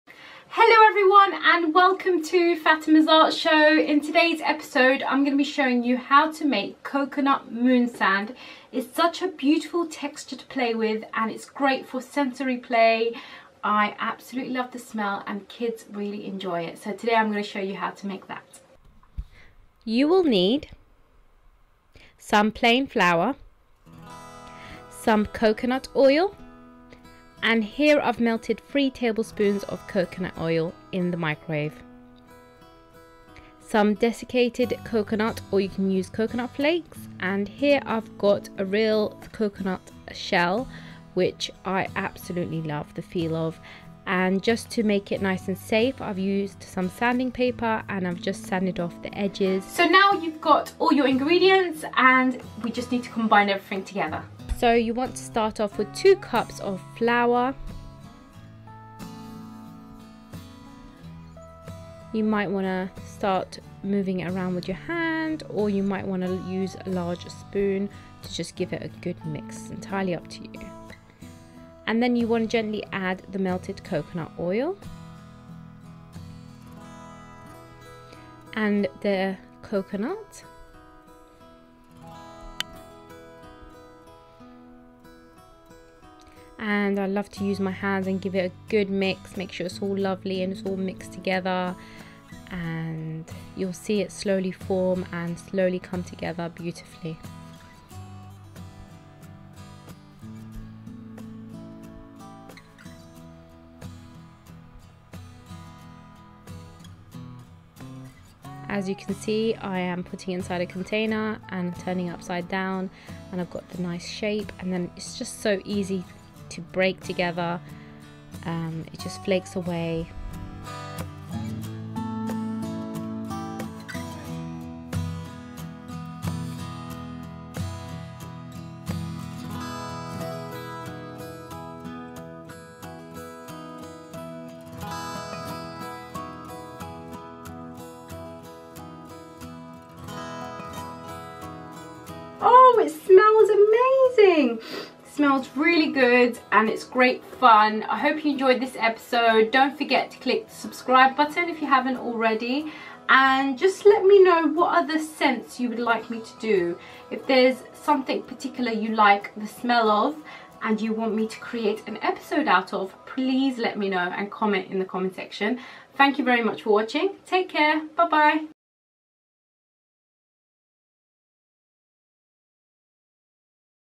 HOW TO MAKE COCONUT MOON SAND! | Fatema's Art Show
How to make coconut moon sand. Today I show to how to create a sensory messy play activity. It's great for toddlers, children and anyone who wants a nice experience playing with coconut scented sand!

Coconut oil - 3 table spoons
Desiccated coconut or coconut flakes
Coconut shell